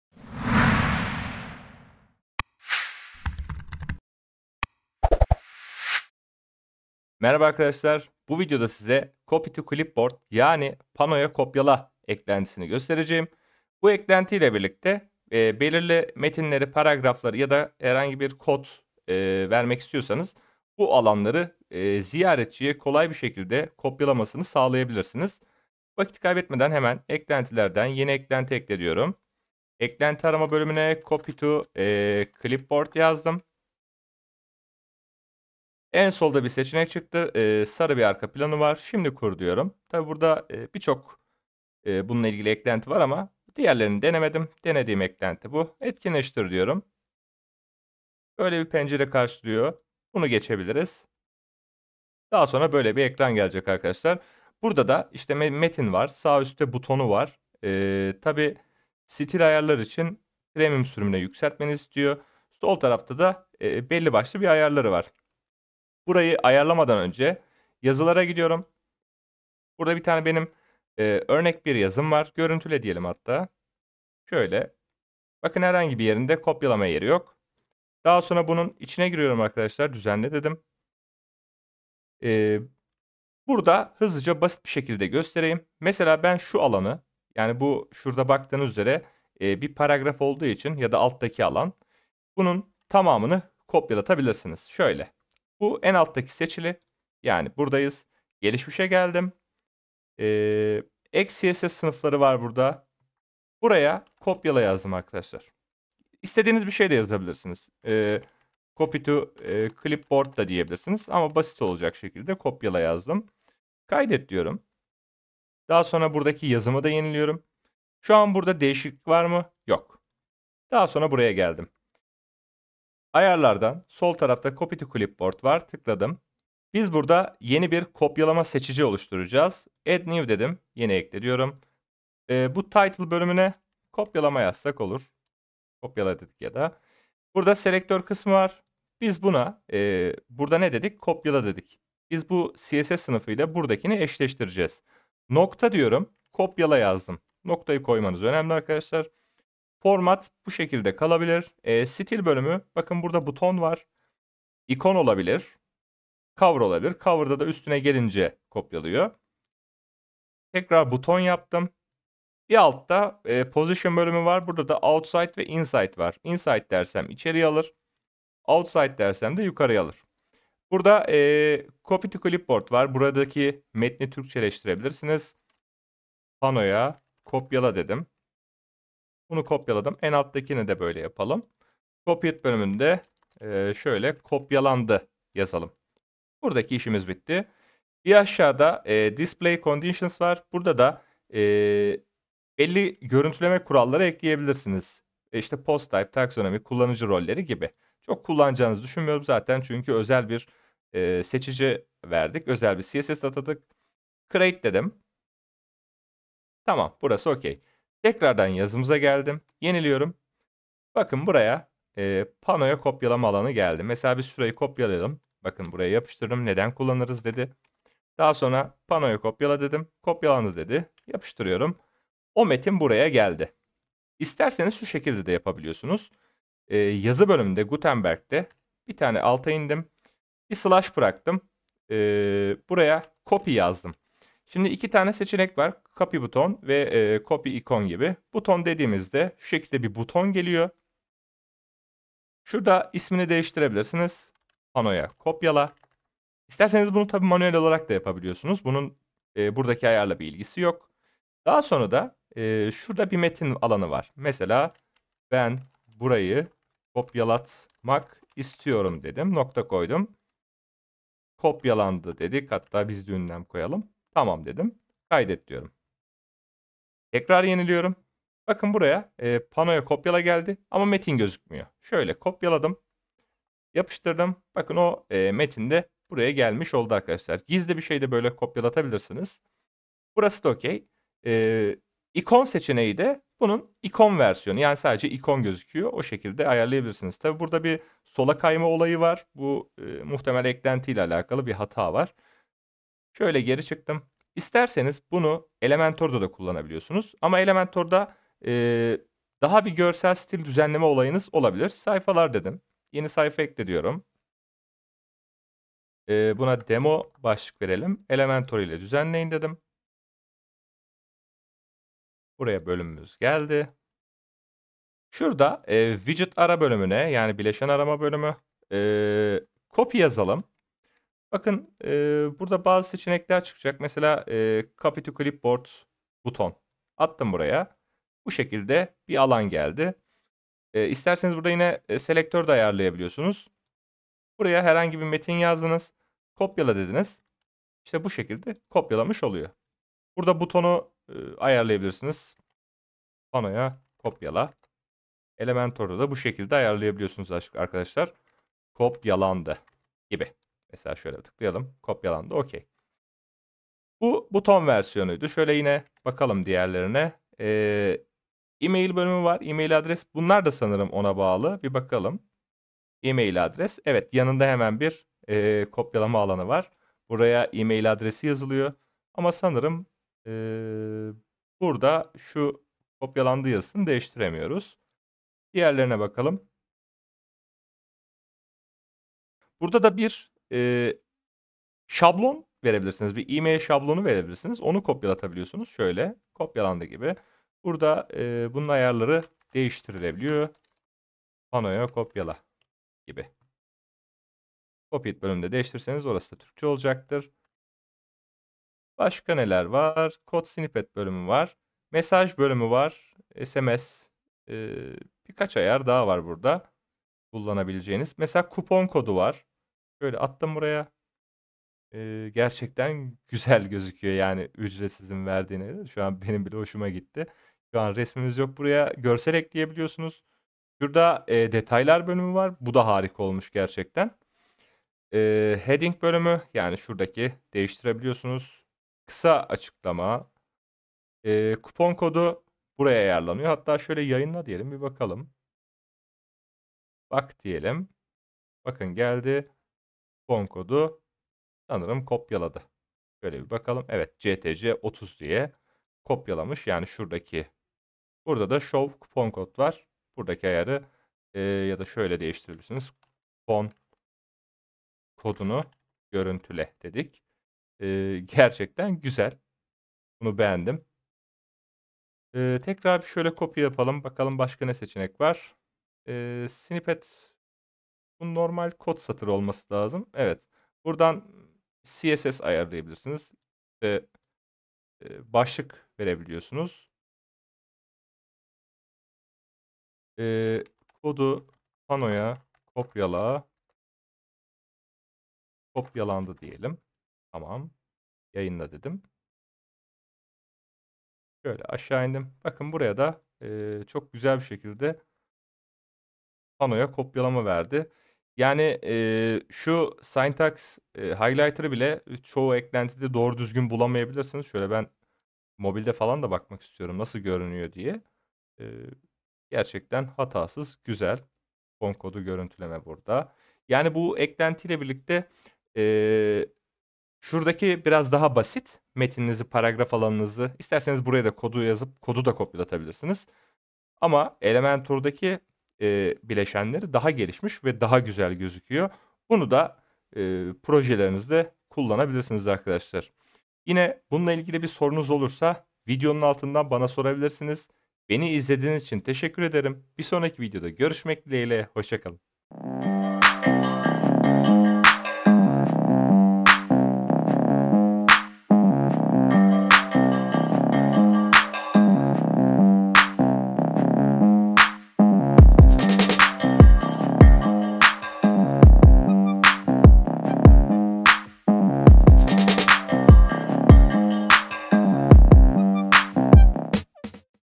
Elementor & WordPress Panoya Kopyala Eklentisi
WordPress gutenberg ve Elementor ile birlikte kullanabileceğiniz panoya kopyalama eklentisi arıyorsanız, bu eklenti tam size göre. Yerleşik gutenberg ile daha basit kopyalama işlevi sağlayan bu eklenti, Elementor ile birlikte çok daha farklı seçenekler sunabiliyor. Bunlardan bazıları;

Kupon Kodu Kopyala,
E-Posta veya E-Posta Şablon Kopyala,
Kod Satırı Kopyala,
SMS Kopyala,
Tablo Kopyala

 gibi seçenekler mevcuttur.

✅ EKLENTİ

✅ OYNATMA LİSTELERİ